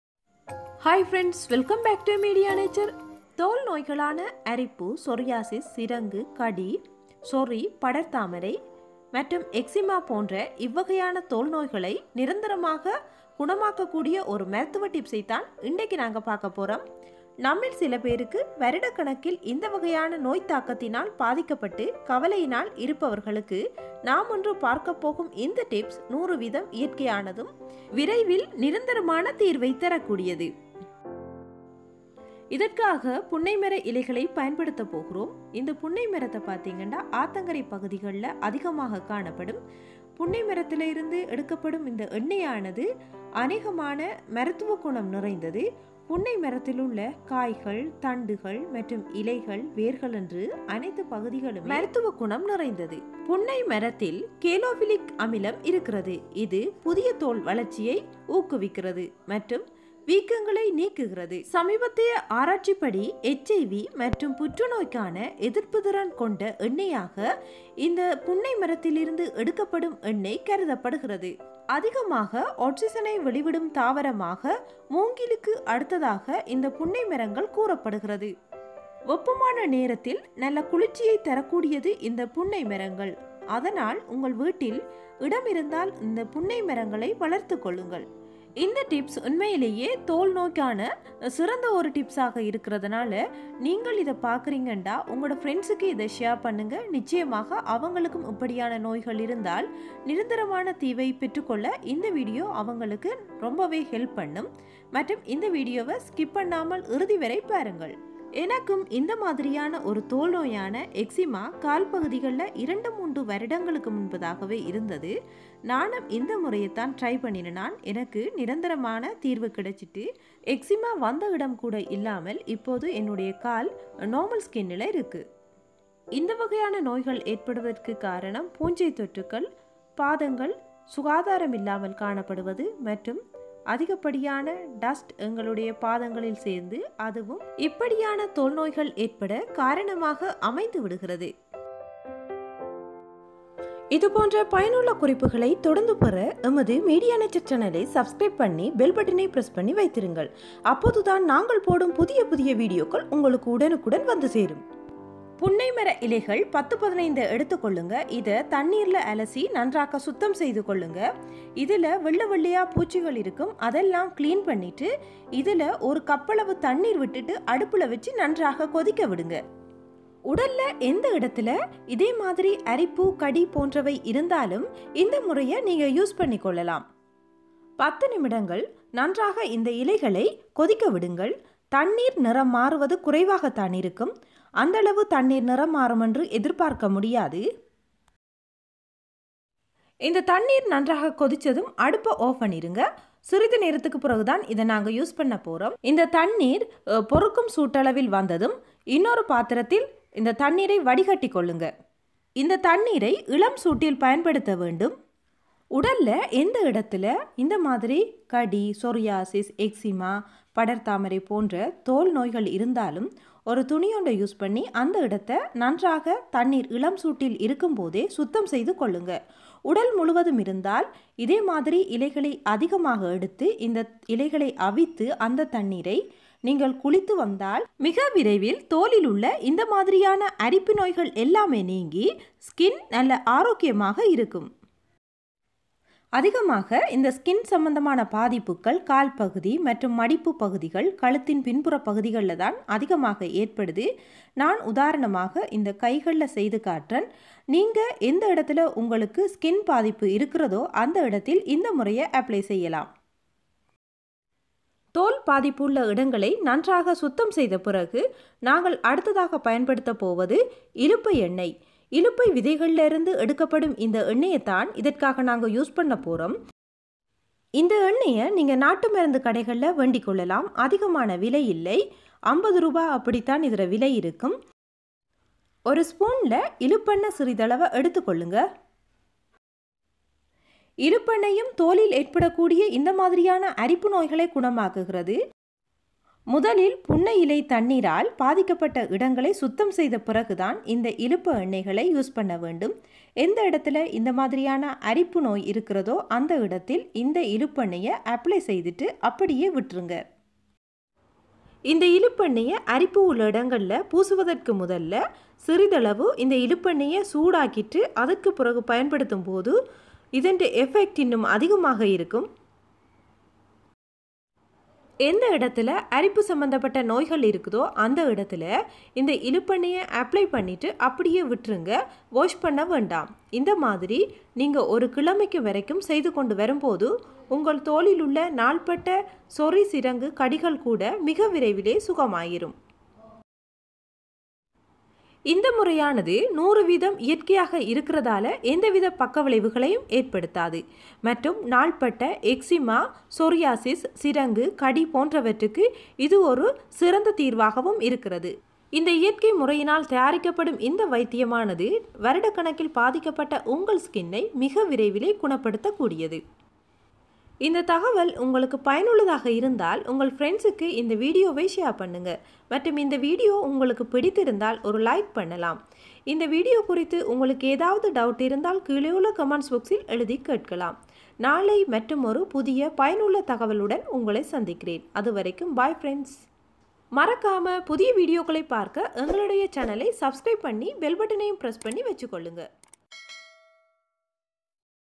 தோல் அரிப்பு | சோரியாசிஸ் | கடி | எக்ஸிமா நிரந்தர தீர்வு | home remedy for Itching in tamil
how to cure skin ichching problem natural homevremoady
இயர்க்சிமுறையில் தோல் சம்மந்தப்பட்ட நோய் நீங்க இந்த முறையை ஒருமுறை செய்துபருபகள்